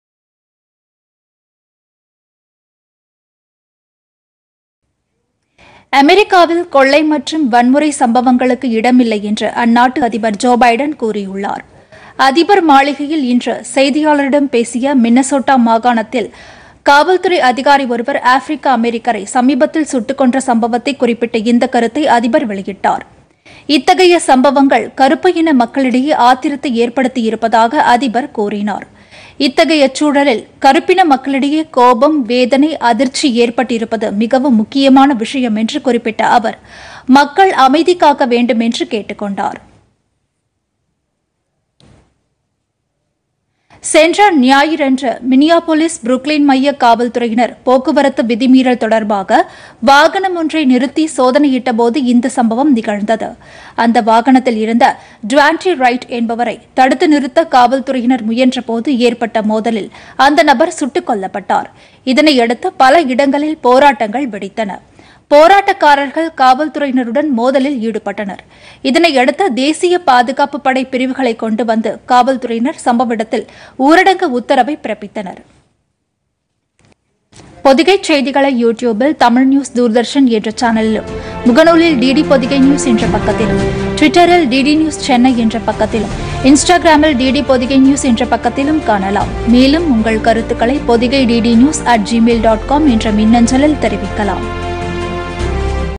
அமெரிக்காவில் வன்முறைக்கு இடமில்லை: ‍ஜோ பைடன்PodhigaiTamilNews
மின்னசோட்டா சம்பவத்தின் பின்னணியில் அமெரிக்காவில் வன்முறைகளுக்கு இடமில்லை - அதிபர் ஜோ பைடன்.